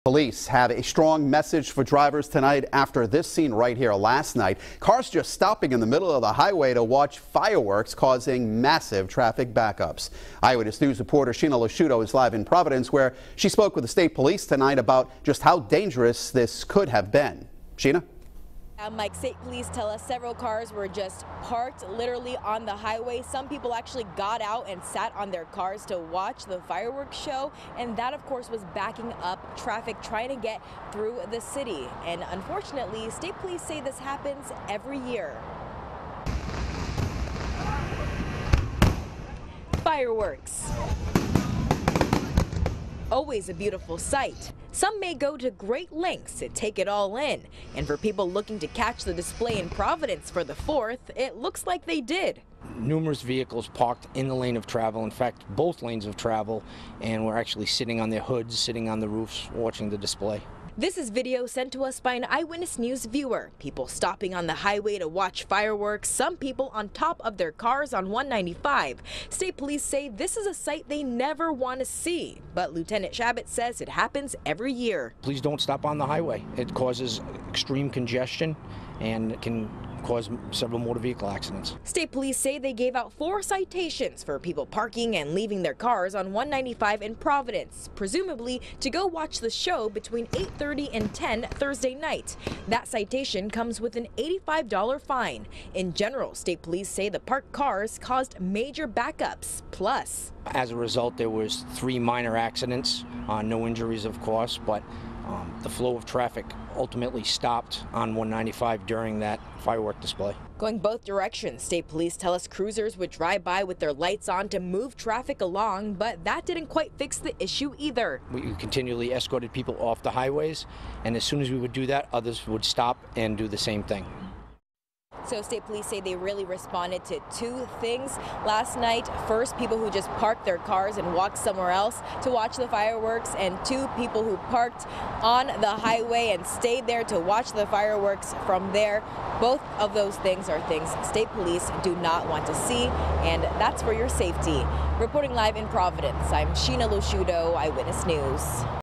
RI State Police: Please don’t park on the highway
The highway spectators caused massive traffic backups in the area - especially after the fireworks show ended and people were trying to go home.